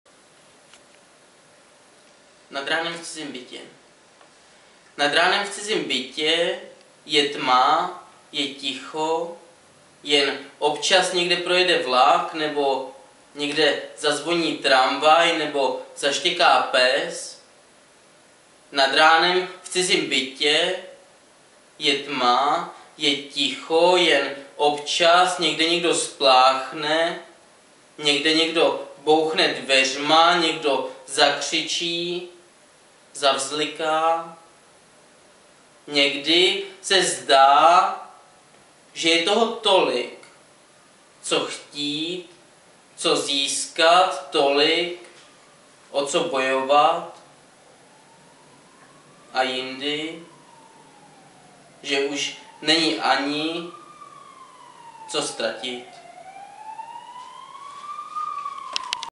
Jan Těsnohlídek ml. - Nad ránem v cizím bytě
Ze sbírky "Ještě je co ztratit" (JT's nakladatelství 2013) pro web Nedělní chvilka poezie.